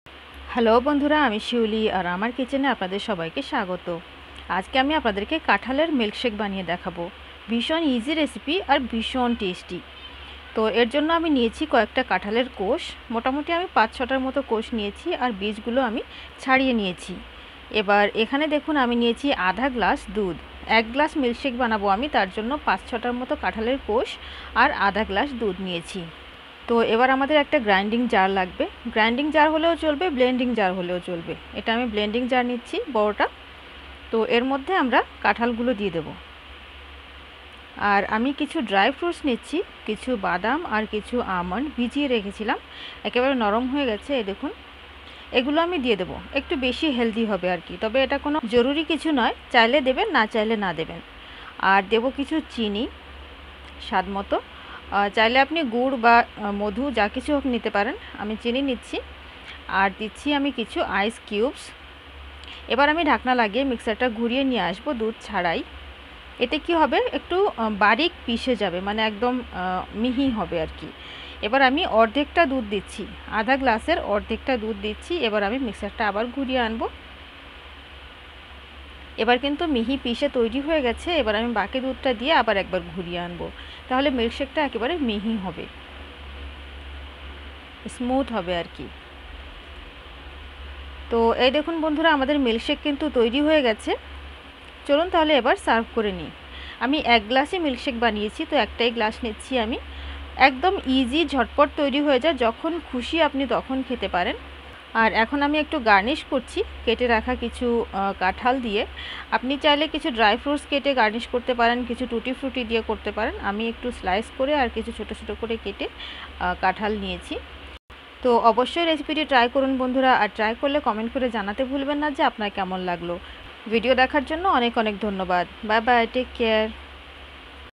কাঠালের মিল্কশেক | Easy and Yummy Jackfruit Milkshake Recipe in বাংলা | Shiuly Gaikwad
shiuly_and_her_kitchen